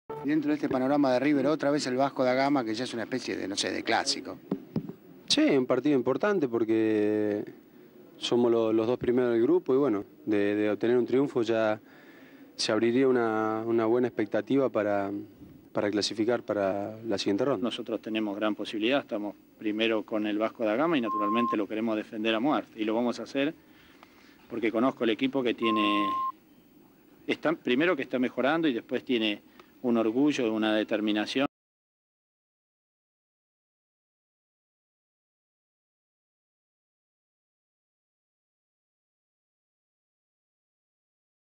Hernan Diaz y Ramon Diaz hablan de partido contra Vasco Da Gama 1998 BG-0579 - DiFilm
CONSULTAS PARA ADQUIRIR VIDEOS A: - FUTBOL: REPORTAJE A HERNAN DIAZ Y AL DIRECTOR TECNICO ANGEL RAMON DIAZ SOBRE PARTIDO CON EL VASCO DA GAMA DE BRASIL POR LA COPA MERCOSUR. INSERT ENTRENAMIENTO DE RIVER PLATE.
Fecha: 16/9/98
Duración:
Código: BG-0579